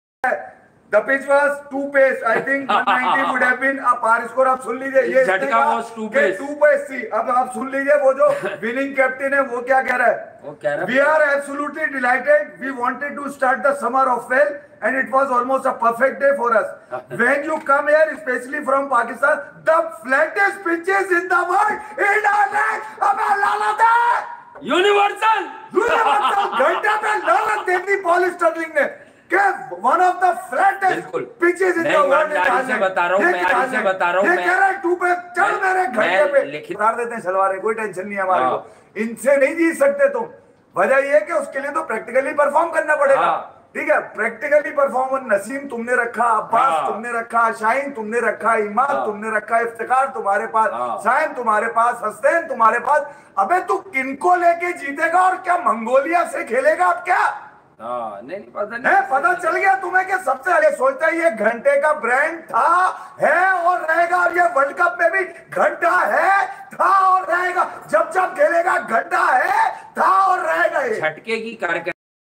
Pakistan Vs Ireland ICC T20 Cricket Match | Wasay Habib Opinion
Pakistan Cricket Match With Ireland With 3 T20i Icc Match

pakistan Loss their 1st T20 icc Match Against Ireland

1/3 Ireland

pakistan Cricket is in Danger Right Now

Poor Batting by BabarAzam , M Rizwan as Well As Bowling

Wasey Habib Opinion About This Match
Video Credit : Wasay Habib